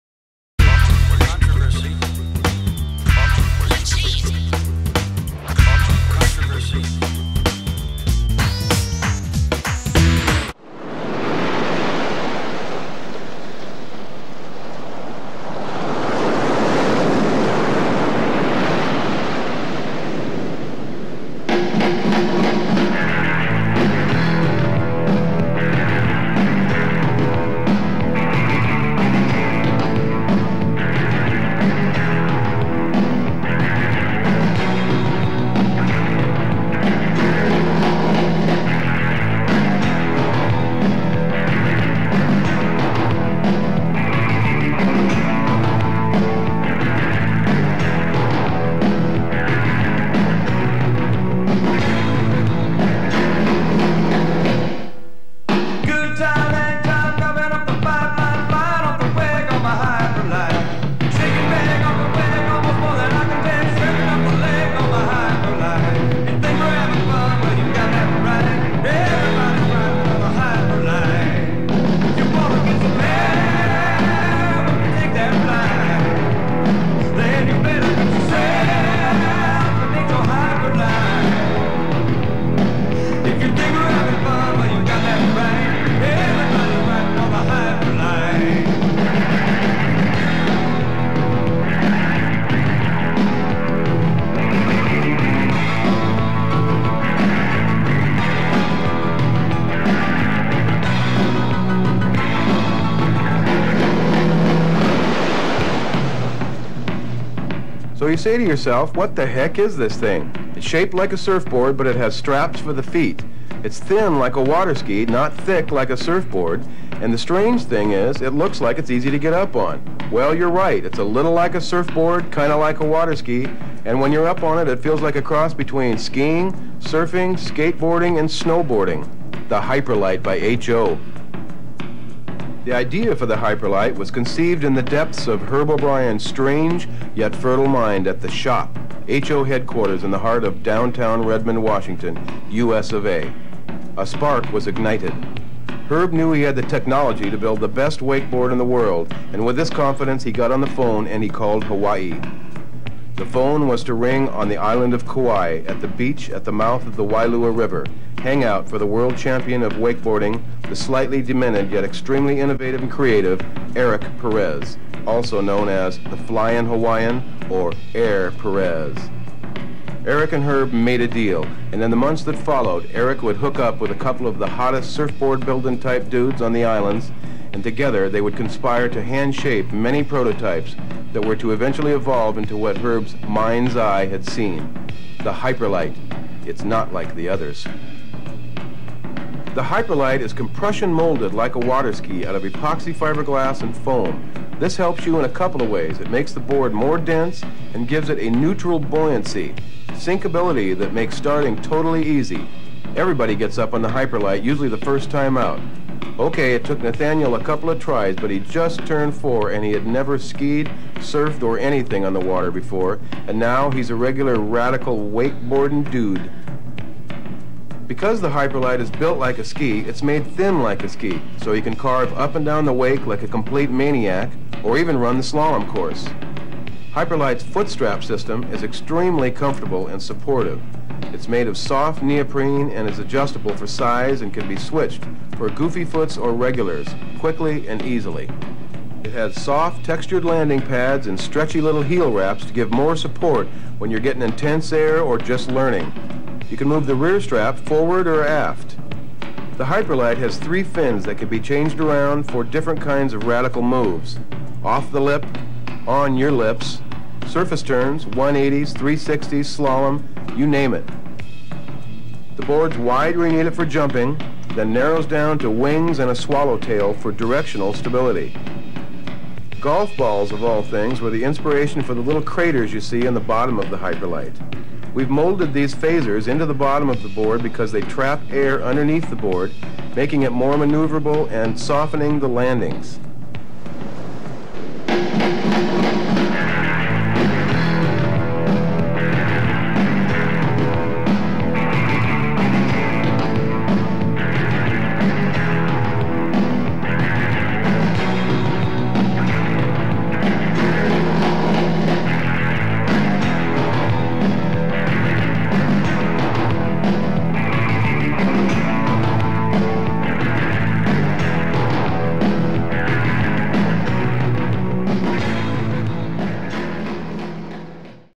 1991 First Hyperlite Wakeboards Promotional Video. Featuring World Champion Eric Perez
Watch the video that started a revolution…
In January 1991 Herb O’Brien unveiled the HO Hyperlite. It was the first compression-molded wakeboard, and the customer response was immediate and dramatic. The thin profile and near neutral buoyancy boards allowed for much easier starts and greatly improved performance. Hyperlite’s first full page ad featured 2-time World Champion Eric Perez with the bold title “WAKEboarding - The HO Hyperlite defines the sport.”
Herb’s new board was such a game changer that in many ways it did redefine the sport. The foam filled floating “SKIBOARDS” of the 1980s gave way to the thin profile, high performance “WAKEBOARDS” in the 1990s and beyond. Herb went on to become the driving force behind a new company Hyperlite wakeboards which continued with innovative products and events to help establish the sport.

FEATURING:
Herb O’Brien: Producer
Eddie Roberts, Jr.: Director, Videographer, Editor, Narrator
Eric Perez: Rider
Nathaniel Fisher: Rider
Jonathan Fisher: Rider
Mike Murphy: Rider
Beth Fisher: Model
Kenny Terheggen: Driver
Dan Morales: Press Operator
Randy Gerber: Music